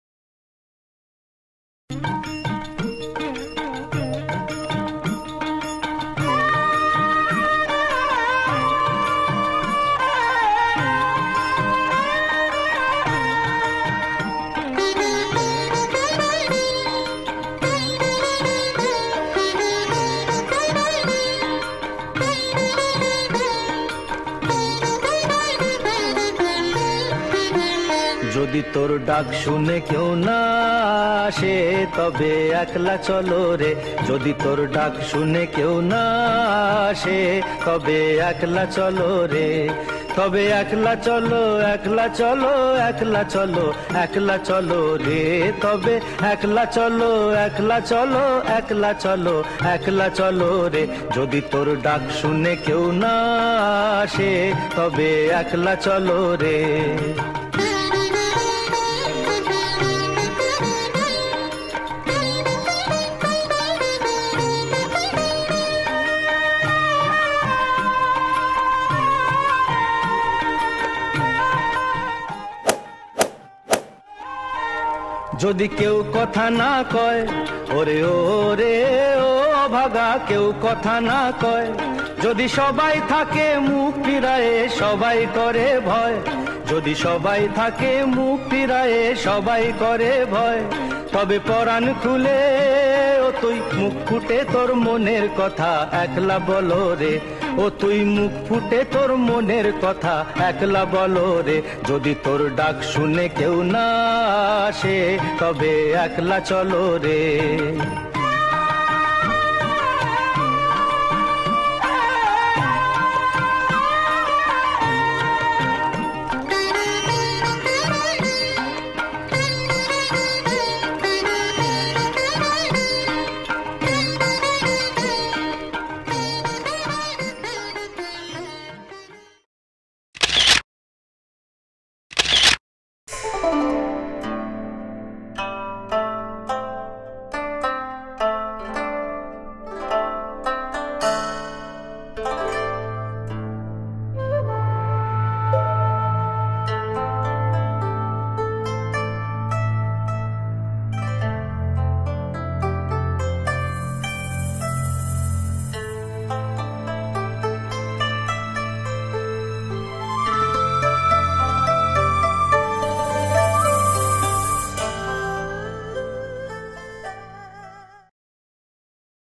Tagore Design Principles
A short film on the design principles that is taught at Breaking Rules School of practical photography in Mumbai.